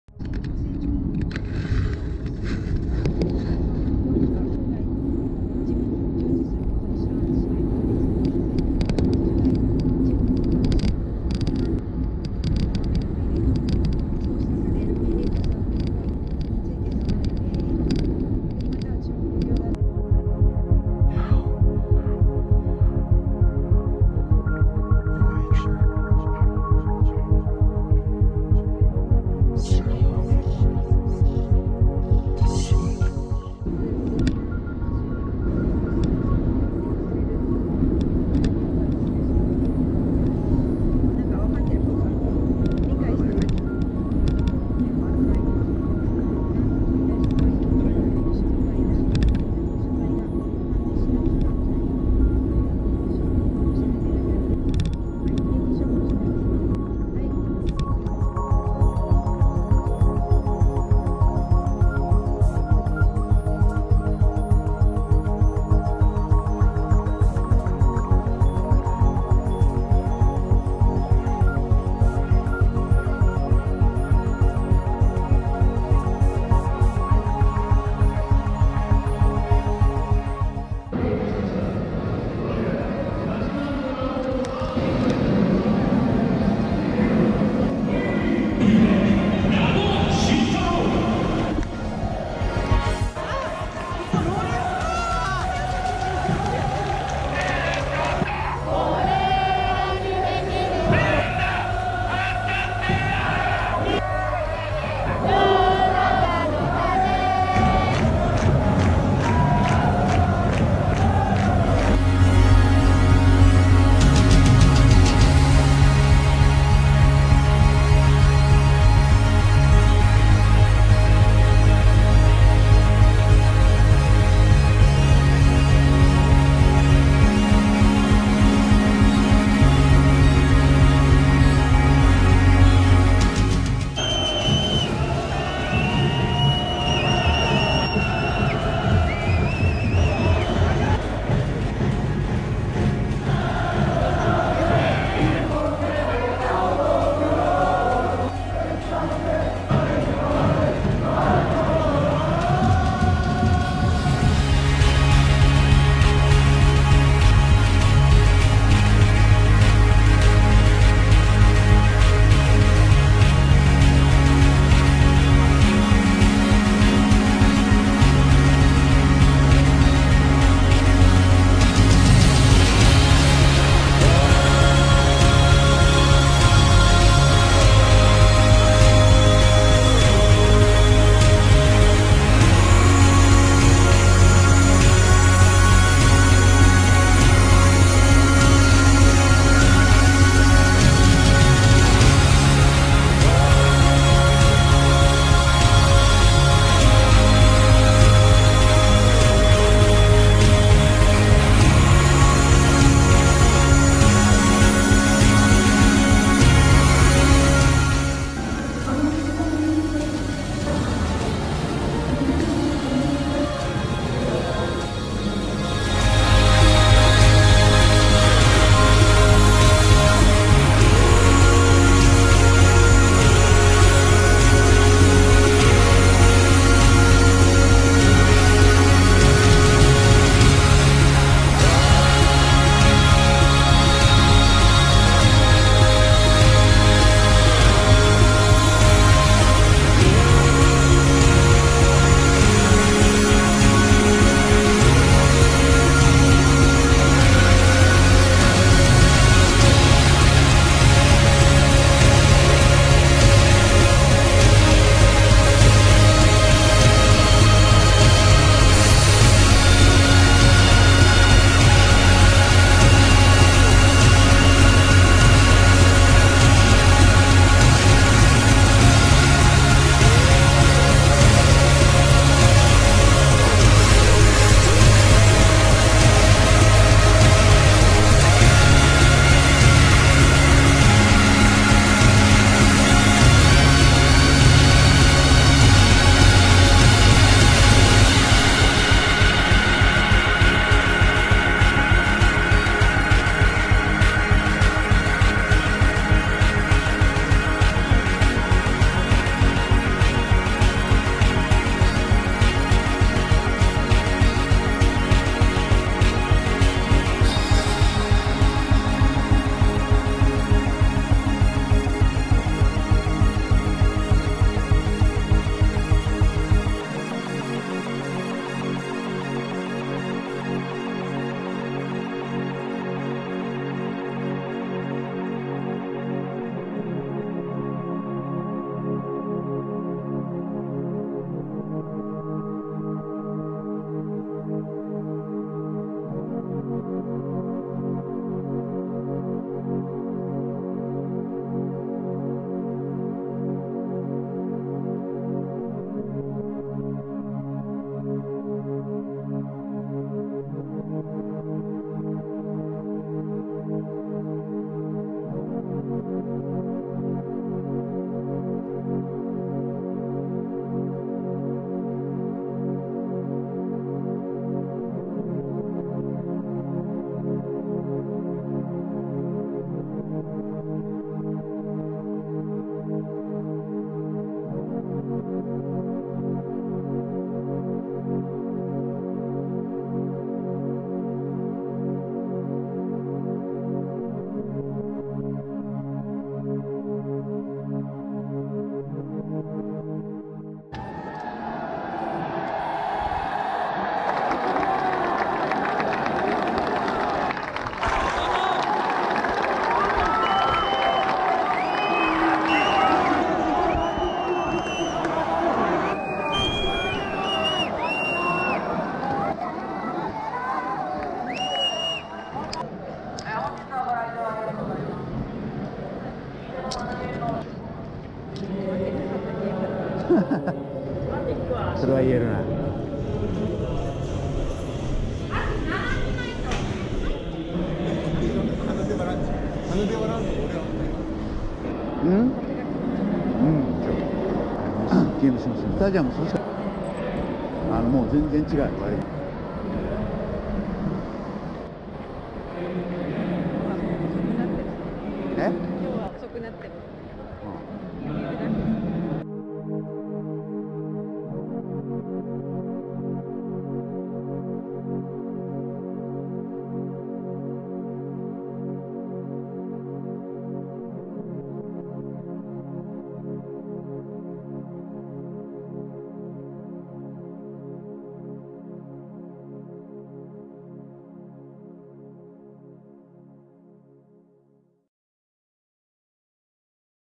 【ガンバ大阪】2019鹿島遠征まとめムービー 【アウェイの美学】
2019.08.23 鹿島サッカースタジアム 平日の遠征はキツイです。